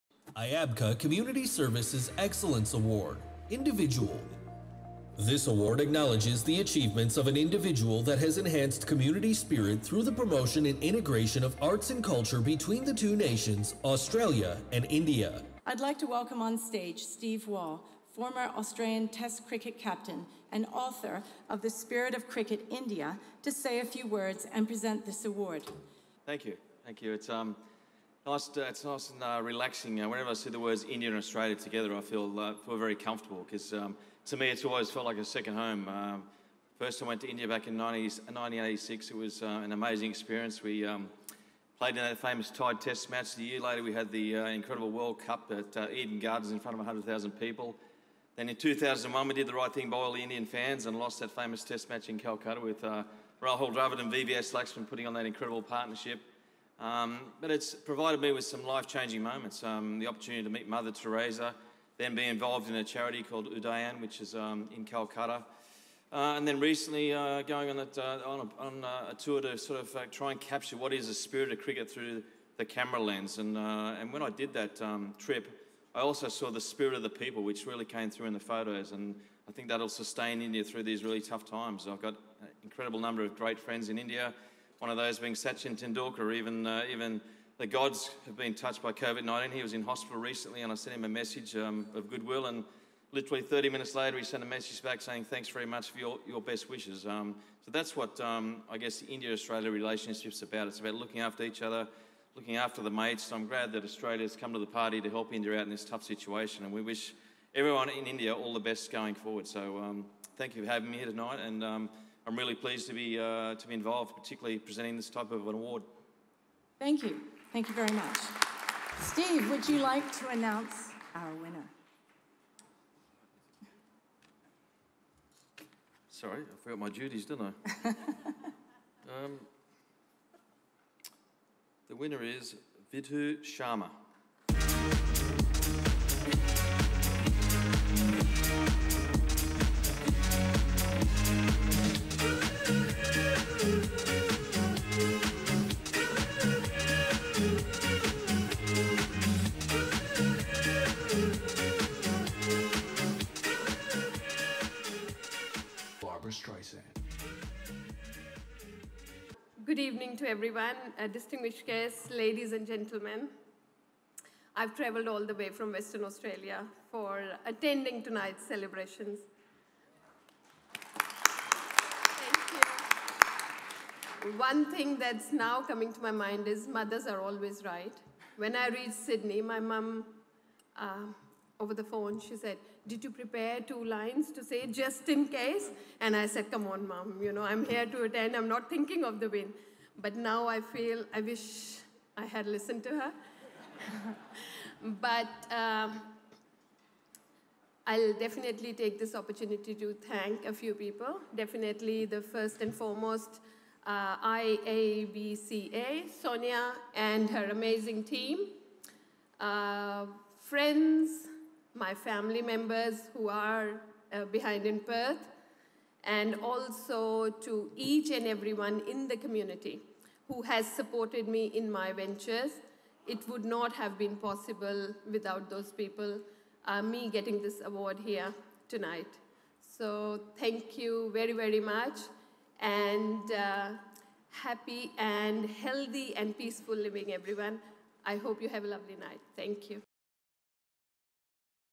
IABCA Community Service Individual Award won by Vidhu Sharma of Ayurveda for Wellness Centre
This award acknowledges the achievements of an individual or organisation/project that has enhanced community spirit through initiatives that empower communities including/ or through the promotion and integration of arts and culture in Australia and/or India. Nominations will be assessed on the below five criteria, each of which are given equal weighting.
* Community engagement and collaboration
*  Events/projects associated with enhancing cultural and community integration
*  Positive impact on the organisation
*  Ability to overcome challenges
*  Implementation and delivery of project/event